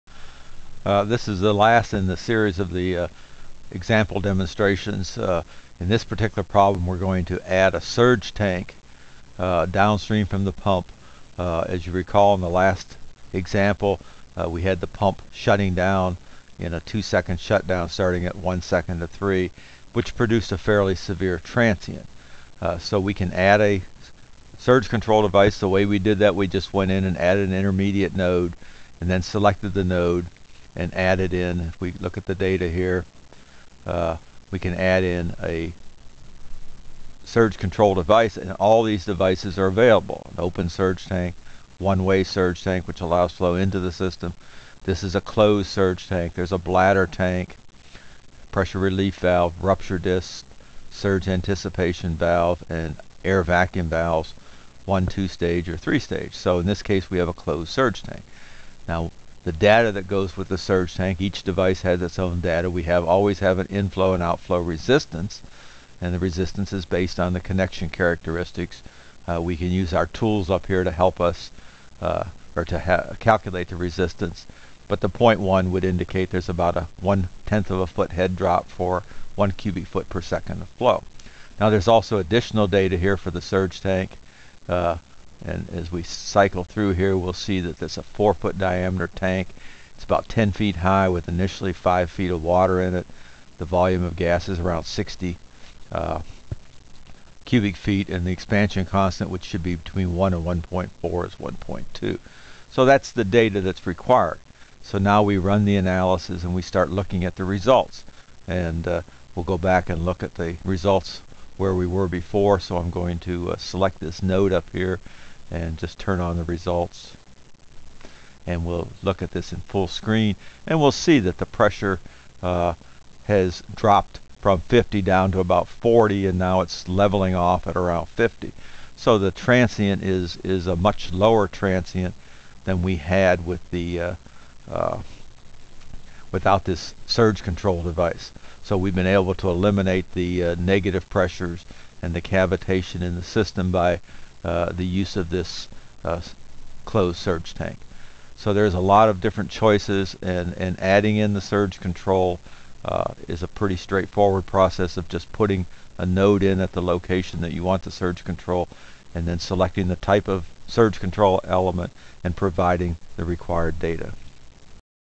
Surge Tanks
Dr. Don J. Wood describes the different types of surge tanks and the data required to deploy an accurate surge model and analysis.